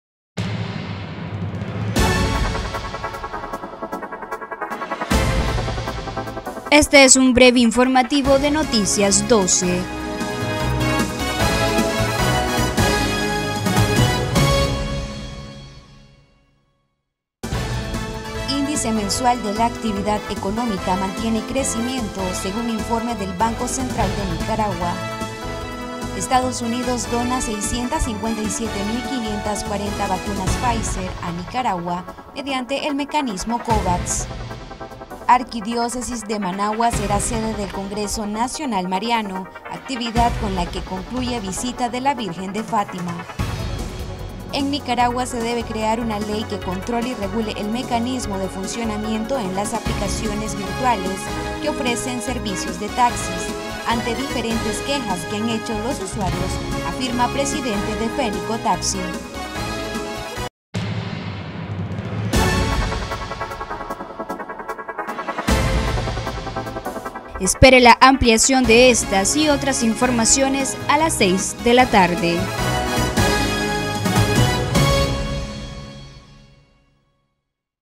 Breve Informativo
📍Índice Mensual de la Actividad Económica mantiene crecimiento según informe del BCN
📍EE.UU dona 657,540 vacunas Pfizer a Nicaragua mediante el mecanismo Covax
📍Arquidiócesis de Managua será la sede del congreso Nacional Mariano
📍Nicaragua debe crear ley que regule y controle las aplicaciones de taxis